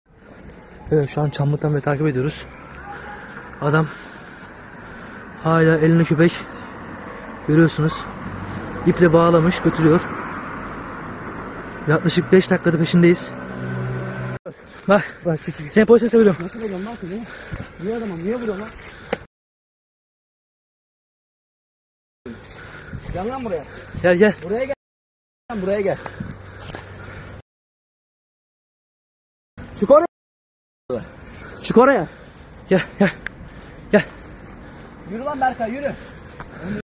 Yaşlı adam köpeğe tecavüz ederken yakalandı... بەهۆی سێکس لەگەڵ سەگێک پیرەپیاوێک دادگایی دەکرێت
پیاوێکی بە تەمەن لە پارێزگای کۆجائەلی تورکیا سەگێک لەگەڵ خۆی دەباتە شوێنێکی چۆڵ.. گەنجانی گەڕەک گومانی لێدەکەن و بەسەریدادەدەن، دەبینن ئەو پیرەپیاوە سێکس لەگەڵ لەگەڵ سەگەکە دەکات.
یەکێک لە گەنجەکان لە پیرە پیاوەکە دەدات و پەیوەندی بە پارێزگاوە دەکات. پارێزگای کۆجائەلی بریاریدا لێپرسینەوەی لەگەڵدا بکرێت و بدرێتە دادگا.
گەنجانی ئەو گەڕەکەکە دەڵێن ئەو پیرە پیاوە دەمێکە ئەو کارە قێزەونە دەکات.
KOCAELİ'nin Derince ilçesinde iple bağladığı köpeği ağaçların bulunduğu bir alana götürerek eliyle cinsel istismarda bulunduğu iddia edilen yaşlı adam, gençler tarafından darbedildi. Görüntülerin sosyal medyada yayınlanmasının ardından Kocaeli Valisi Seddar Yavuz, gereken yasal işlemin yapılacağını bildirdi.
Derince?de boynuna ip bağladığı sokak köpeğini götüren yaşlı kişiyi gören gençler, şüphelenerek takibe başladı. Köpeği ağaçların bulunduğu alana götüren kişi elle cinsel istismarda bulununca gençler tepki göstererek darbetti. Görüntüler sosyal medyada paylaşılırken, vatandaşlar tepki gösterdi.
Ekipler olayla ilgili soruşturma başlattı.